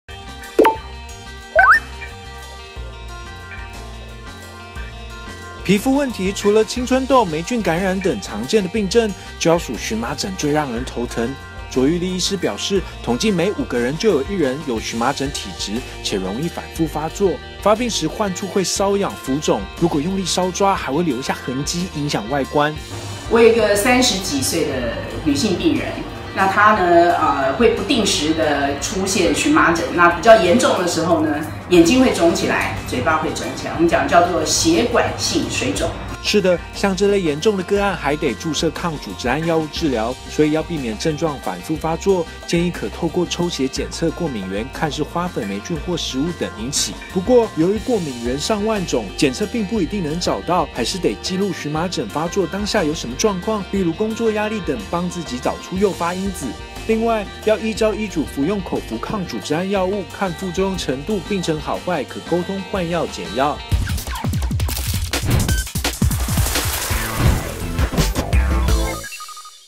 兩成人有這病 癢到受不了 | 台灣蘋果日報
皮膚問題除了青春痘、黴菌感染等常見的病症，就要屬蕁麻疹最讓人頭痛，據流行病學統計，每5個人就有1人曾罹患蕁麻疹，開業皮膚科診所院長卓玉麗指出，蕁麻疹急性發作時，患處會紅癢、微腫，部分人會刺痛，有的過段時間會消失，若症狀持續超過6周，則屬慢性蕁麻疹，無論急性慢性都易反覆發作，需使用抗組織胺藥物控制病情。